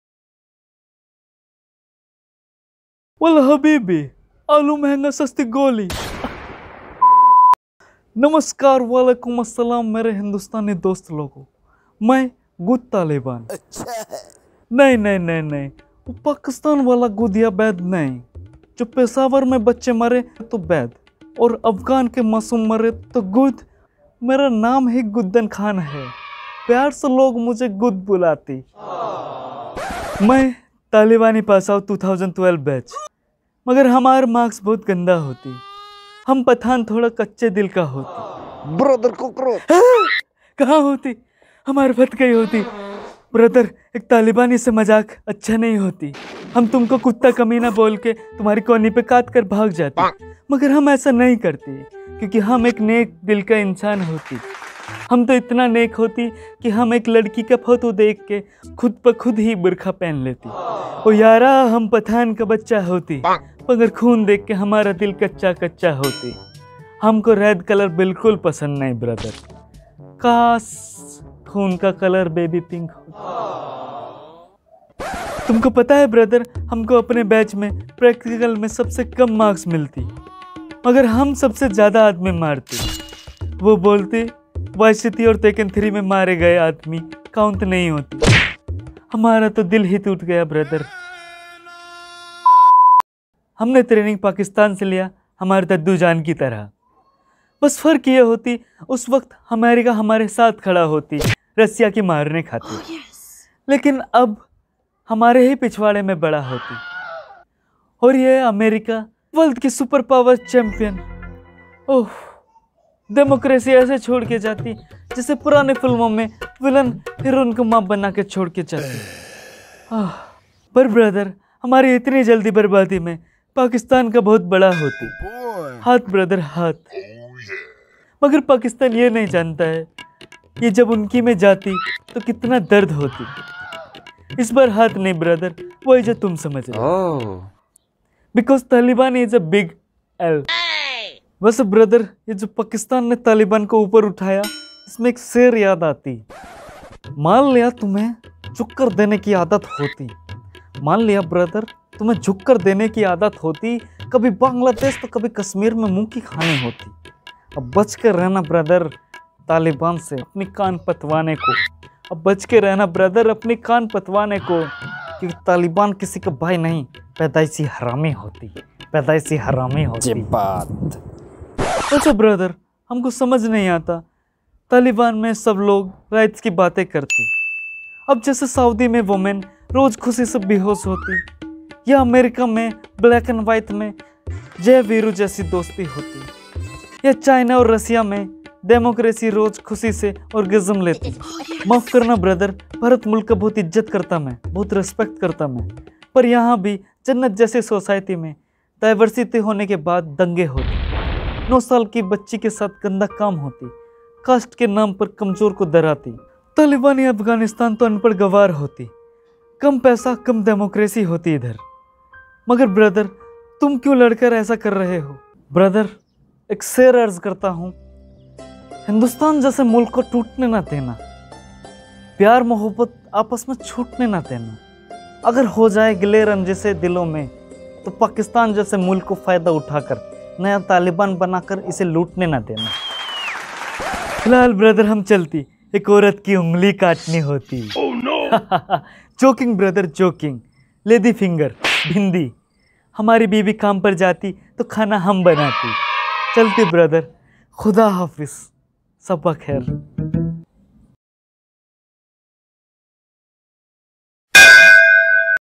Kahani Ek Cute Talabani Ki || Taliban funny video || Thesocialcomedians | TSCs TSC || Viral Comedy
Video gives a funny scenario what if a talibani failed to posses the qualifications of talibani terrorist ? This is satirical comedy to show the reality of the world. Enjoy both the fun and message of video.😇